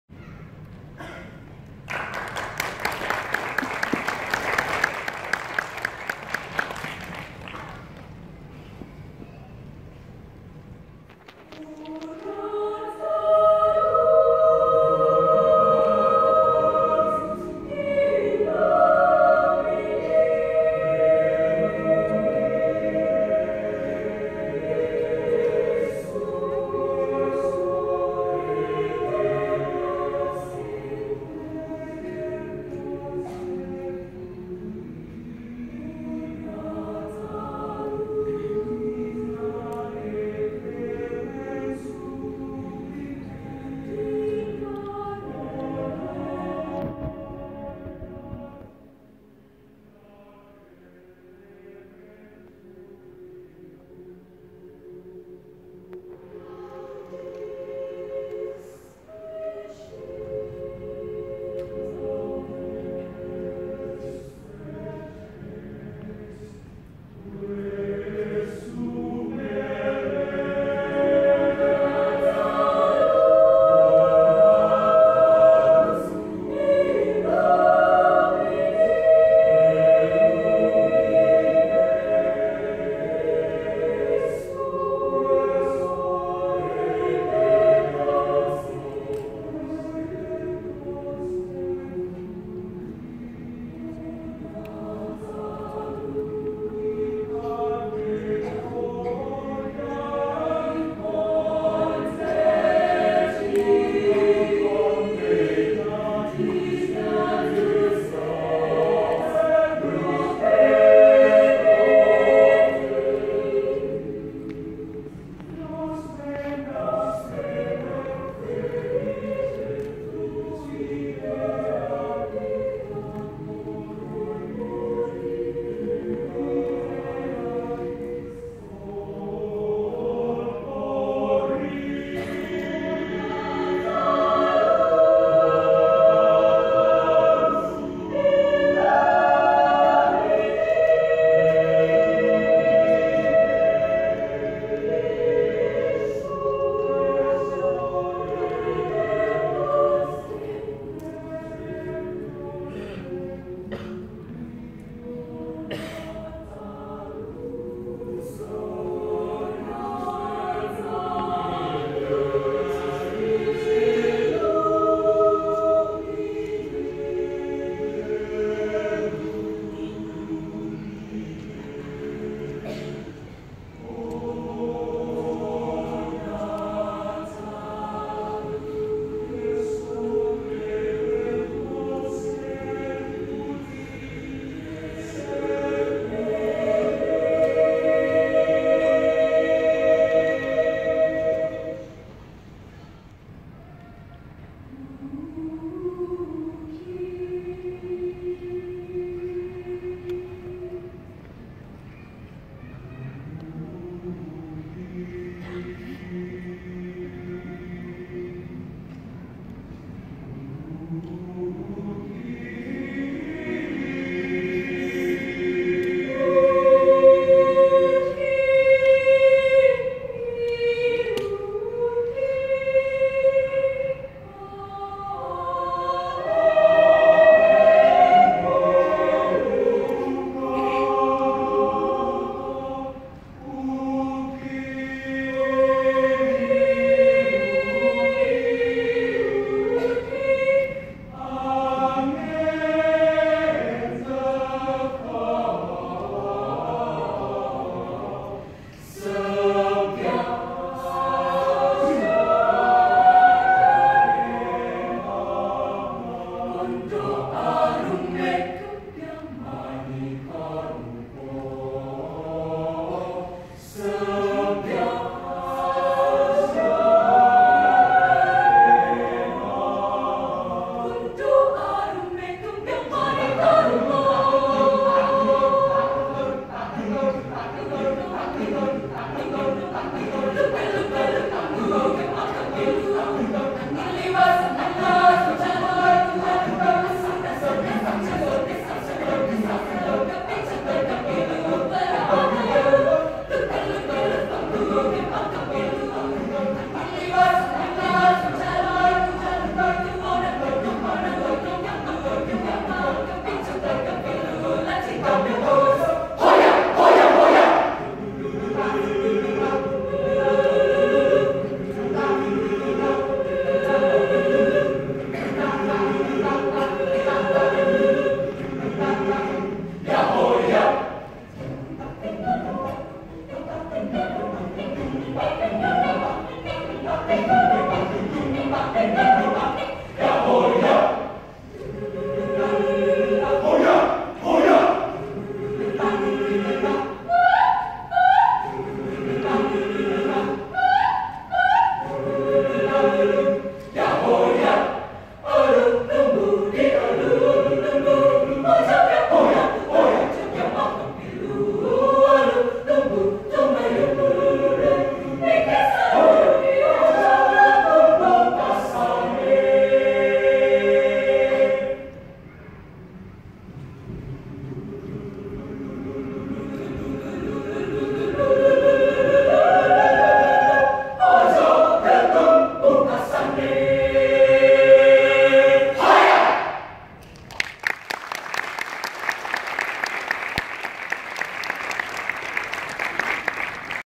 Cantate Choir (SMAK PENABUR Harapan Indah) in BICF 2018
Performance of Cantate Choir (SMAK PENABUR Harapan Indah) in Bali International Choir Festival (BICF) 2018, 24-28 July 2018.

Cantate Choir earned Gold Medal